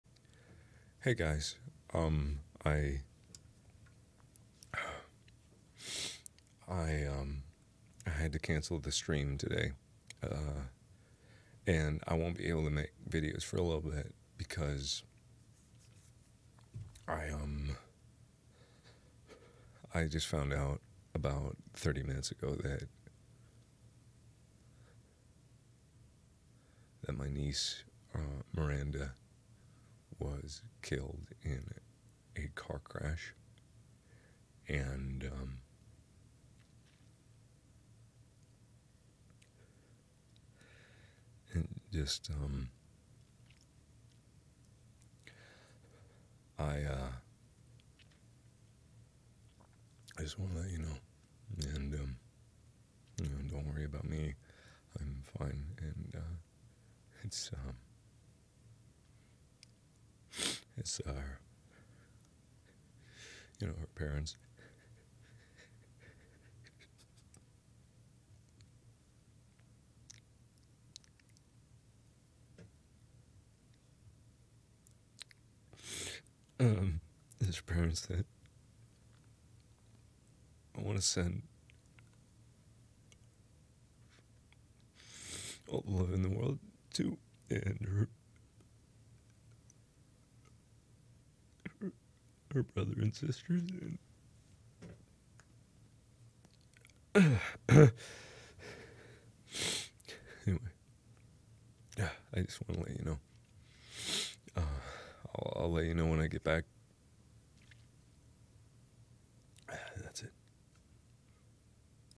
update
Don’t feel obligated to give, only contribute if you have a few dollars to spare. Thanks for all the love and support you’ve shown, the family appreciates it to no end.

I won't be able to upload for a while. Letting you know.